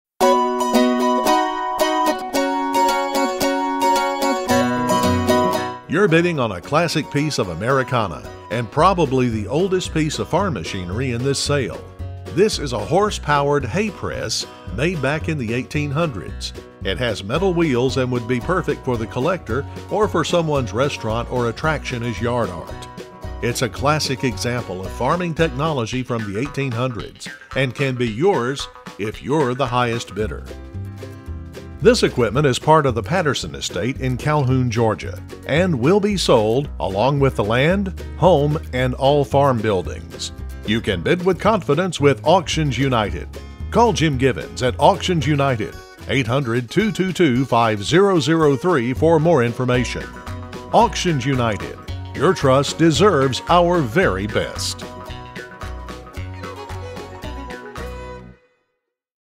Patterson Estate- Lot 33- Antique Horse Powered Hay Press
You're bidding on a classic piece of Americana and probably the oldest piece of farm machinery in this sale. This is a horse powered hay press made back in the 1800's. It has metal wheels and would be perfect for the collector or for someone's restaurant or attraction as yard art. It's a classic example of farming technology from the 1800's and can be yours is you're the highest bidder!

This equipment is part of the Patterson Estate in Calhoun, GA and will be sold along with the land, home, and all farm buildings. You can bid with confidence with Auctions United. Auctions United... Your Trust deserves our very best!